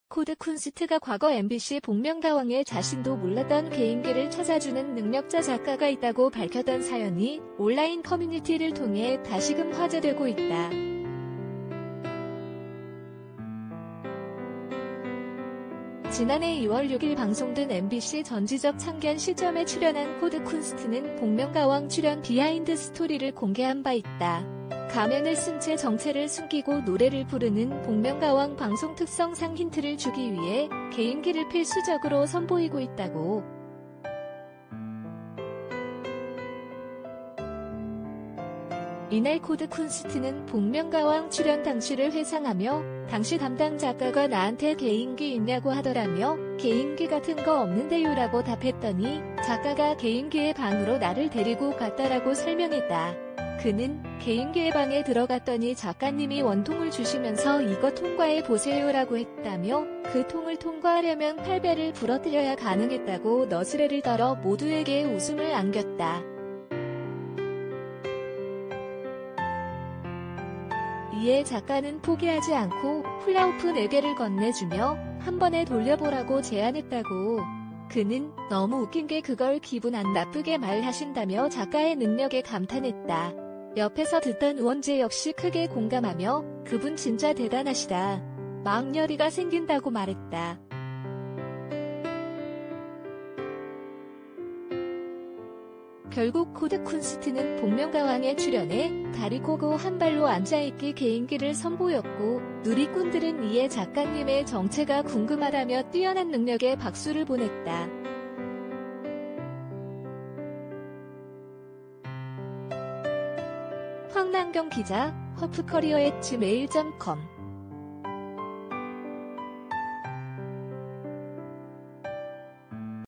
“작가님이 원통을 주면서...” 연예인 사이에서 소문났다는 복면가왕 작가의 비범한 능력은 나도 찾아가 의뢰하고 싶다
코드쿤스트가 과거 MBC ‘복면가왕’에 자신도 몰랐던 개인기를 찾아주는 능력자 작가가 있다고 밝혔던 사연이 온라인 커뮤니티를 통해 다시금 화제 되고 있다.
지난해 2월 6일 방송된 MBC ‘전지적 참견 시점’에 출연한 코드쿤스트는 ‘복면가왕’ 출연 비하인드 스토리를 공개한 바 있다. 가면을 쓴 채 정체를 숨기고 노래를 부르는 복면가왕 방송 특성 상 힌트를 주기 위해 개인기를 필수적으로 선보이고 있다고.
이날 코드쿤스트는 복면가왕 출연 당시를 회상하며 “당시 담당 작가가 나한테 개인기 있냐고 하더라”며 “‘개인기 같은 거 없는데요’라고 답했더니 작가가 ‘개인기의 방’으로 나를 데리고 갔다”라고 설명했다. 그는 “개인기의 방에 들어갔더니 작가님이 원통을 주시면서 ‘이거 통과해 보세요’라고 했다”며 “그 통을 통과하려면 팔 뼈를 부러뜨려야 가능했다”고 너스레를 떨어 모두에게 웃음을 안겼다.
이에 작가는 포기하지 않고 훌라후프 4개를 건네주며 한 번에 돌려보라고 제안했다고. 그는